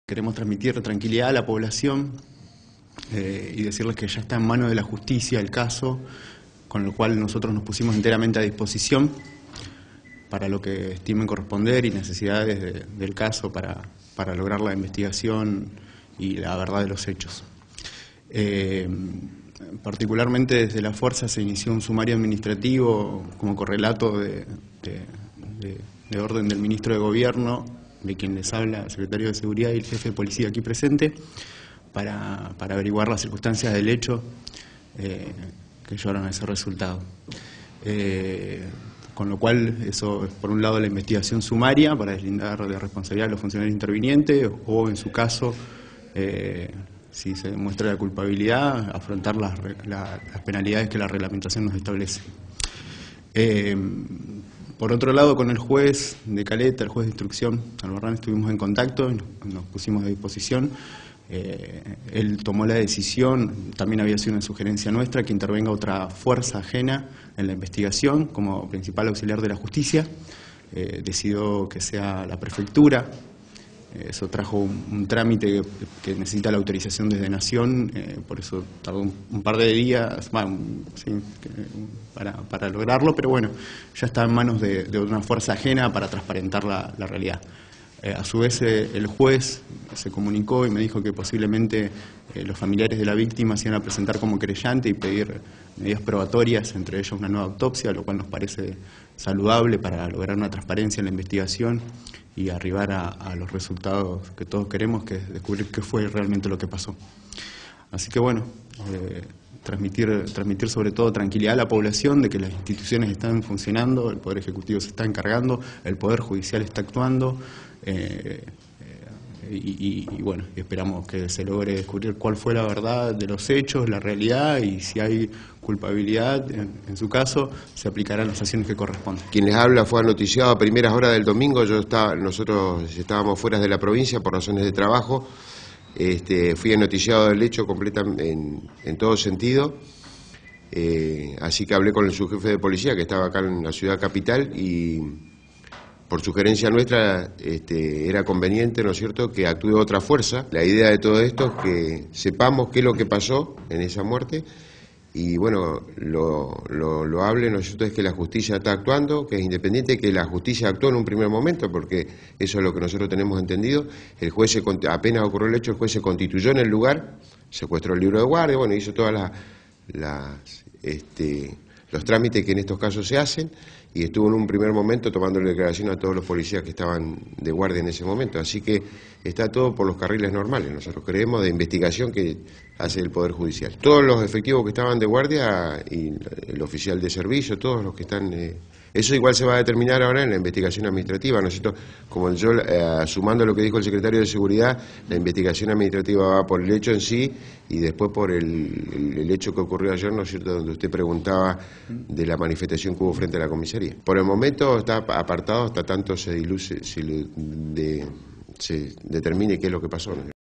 JOSÉ LUIS CORTEZ- LISANDRO DE LA TORRE- SITUACIÓN EN CALETA OLIVIA POR CASO GEREZ
UN VECINO DE LA LOCALIDAD DE CALETA OLIVIA MURIÓ DE FORMA DUDOSA EN EL INTERIOR DE UNA COMISARÍA, LO QUE GENERÓ UNA MASIVA MOVILIZACIÓN DE VECINOS QUE PIDEN JUSTICIA PARA DETERMINAR DE QUE MANERA PERDIÓ LA VIDA EL CIUDADANO DE APELLIDO GEREZ.
EN ÉSTA MASIVA MARCHA QUE SE REALIZÓ AYER HUBO INCIDENTES Y ALGUNOS VECINOS RESULTARON HERIDOS TRAS CORRIDAS Y HECHOS DE VIOLENCIA QUE SE GENERARON CON LA POLICÍA.
VECINOS Y FAMILIARES APUNTAN A QUE LA VÍCTIMA FUE ASESINADA POR LA FUERZA.
EN ÉSTE MARCO, SE REALIZÓ EN HORAS DE LA MAÑANA EN LA JEFATURA DE POLICÍA, UNA RUEDA DE PRENSA EN LA CUAL EL JEFE DE LA FUERZA PROVINCIAL JOSÉ LUIS CORTEZ Y EL SECRETARIO DE ESTADO DE SEGURIDAD INFORMARON EN QUE ESTADO SE ENCUENTRA LA CAUSA.